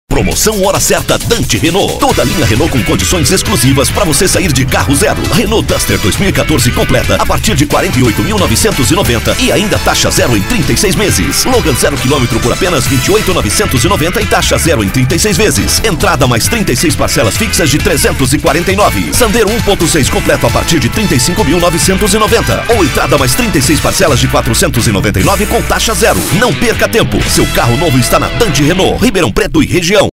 Promoção Hora Certa Dante Renault
Na Dante, toda a linha Renault com condições exclusivas para o cliente adquirir seu carro zero.
Campanha promocional criada pela LabCom Comunicação Total.